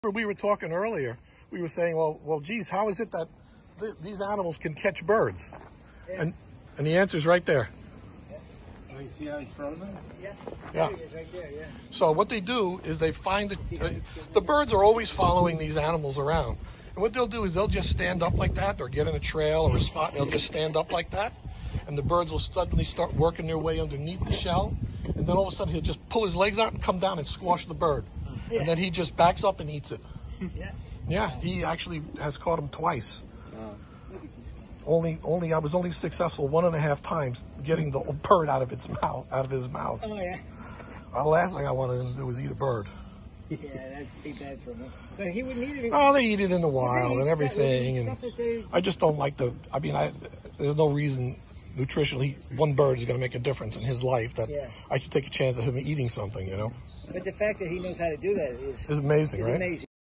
How Giant Tortoises catch Birds.
It may seem impossible but this video explains how giant tortoises can catch birds🐢🦅